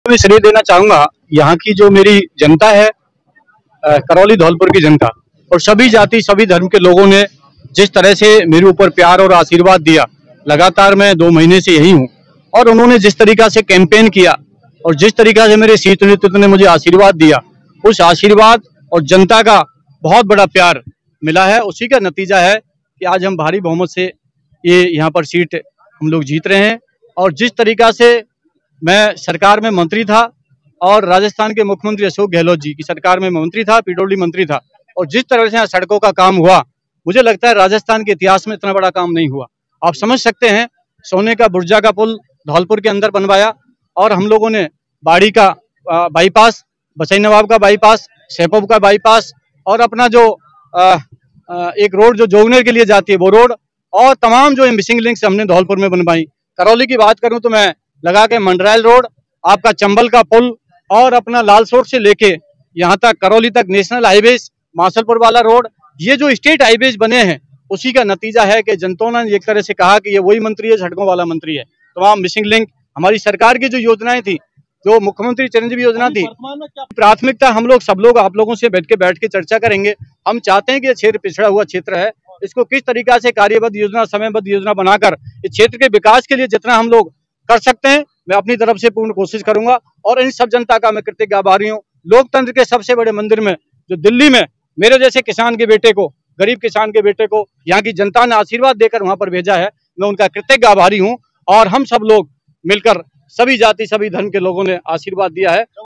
Dholpur Karauli से congress प्रत्याशी Bhajan Lal jatav ने जीत के बाद दिया बयान | loksabha election
Dholpur Karauli से Congress प्रत्याशी Bhajan Lal jatav ने जीत के बाद दिया बयान | loksabha election 2024 | political panchayat | bjp

Rajasthan Lok Sabha Chunav Result 2024: राजस्थान (Rajasthan Lok Sabha Election Result) के धौलपुर करौली (Dholpur Karauli Lok Sabha Seat) से कांग्नेस प्रत्याशी (Congress) भजनलाल जाटव (Bhajan lal Jatav) ने  बंपर जीत हासिल की है . इनका सामना  भाजपा  के इंदू देवी  (Indu Devi) से था. भजनलाल जाटव ने जीत के बाद अपनी प्रतिक्रया दी.
rajasthan election updates
today news updates
barmer election updates